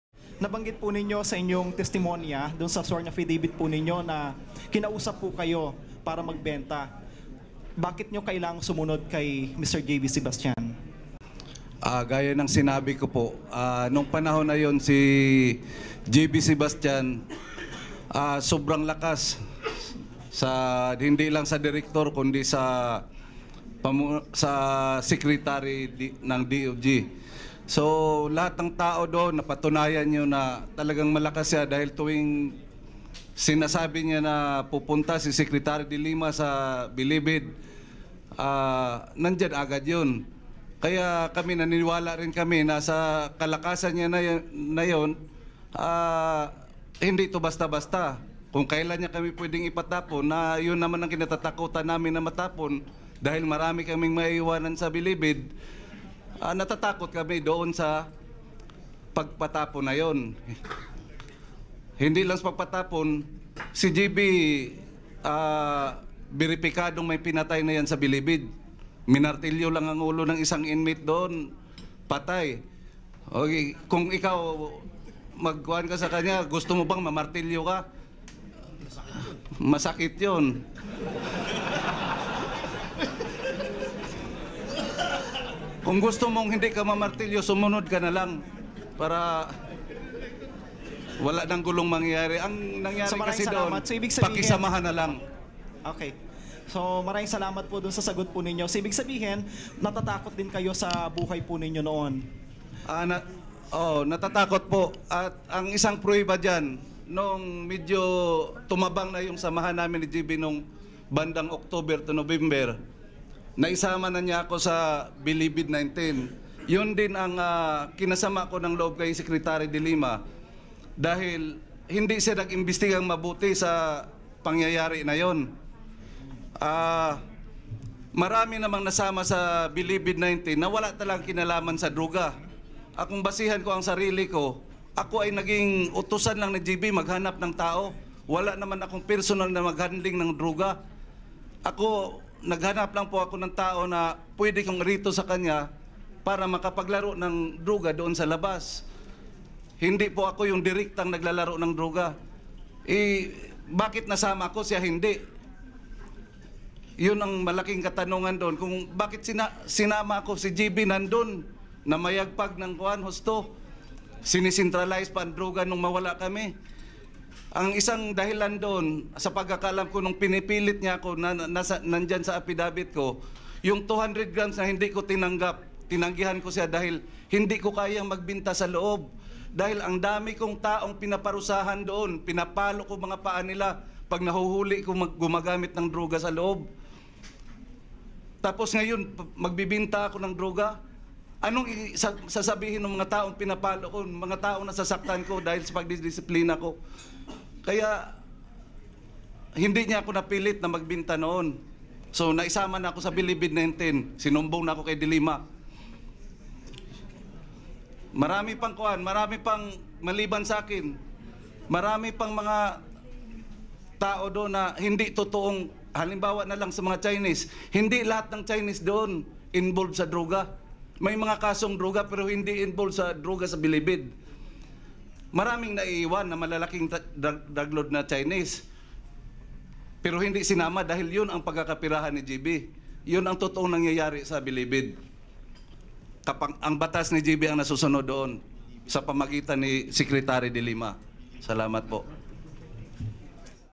‘Jaybee Sebastian’s law ruled Bilibid’
More videos on Rappler Follow Rappler for the latest news in the Philippines and across the globe. Inmate Noel Martinez says Jaybee Sebastian had his way inside the high-security prison because of former justice secretary Leila de Lima. Full story